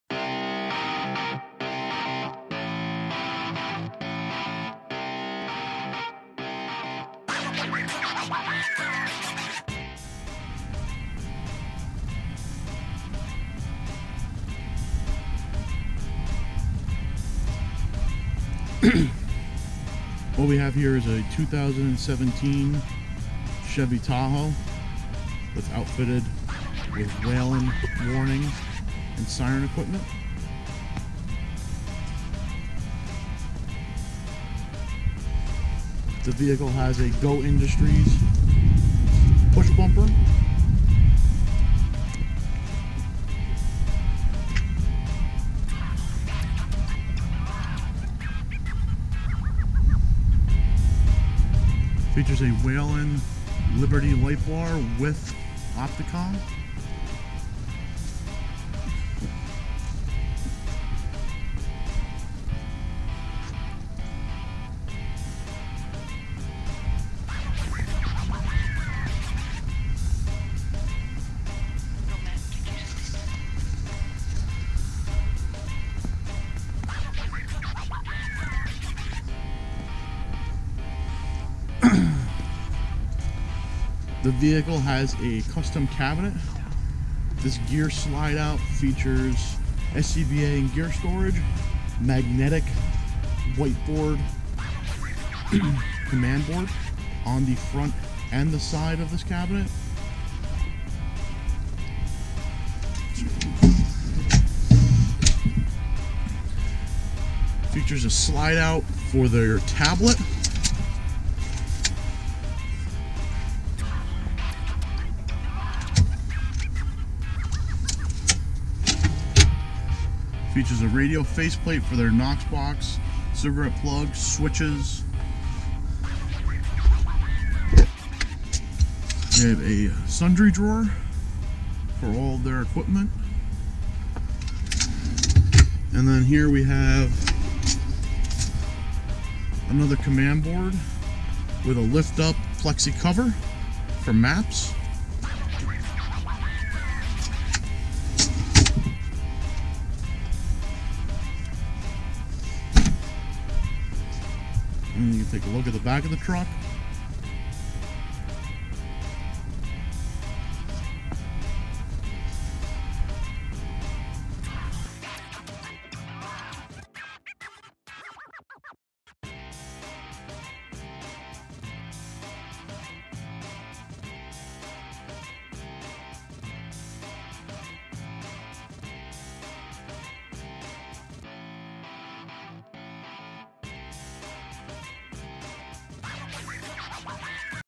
Franklin Township Fire District #1 Car 44 2017 Tahoe Command Vehicle
The Franklin Township Fire District #1 worked with us to design and manufacture their new command vehicle. Featuring Whelen Carbide Siren/Light Controller, custom console, and custom cabinet.